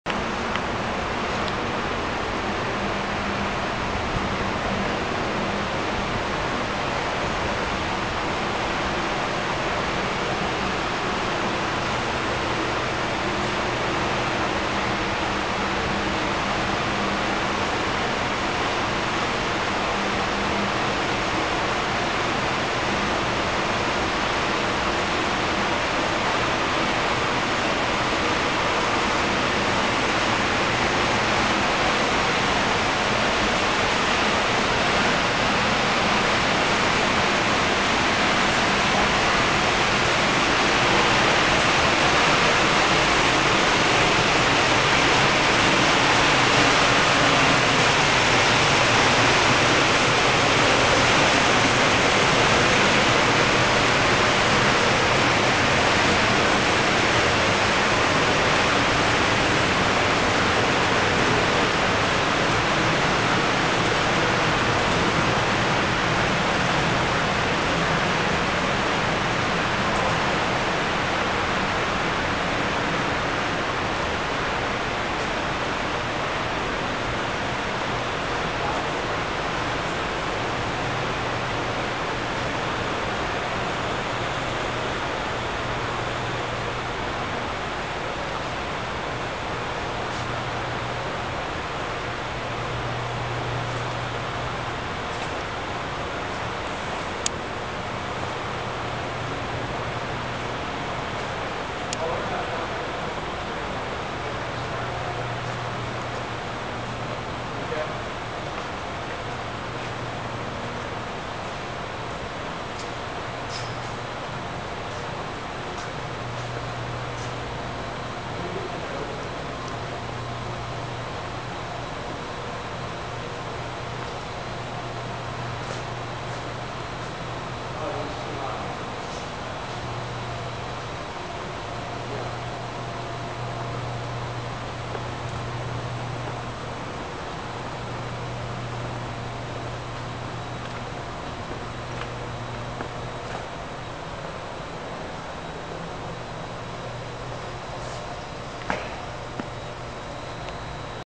Abound line 2 closer view
This is a video traveling up the second Abound PV production line facility. It was sold off at auction the week of October 1st 2012.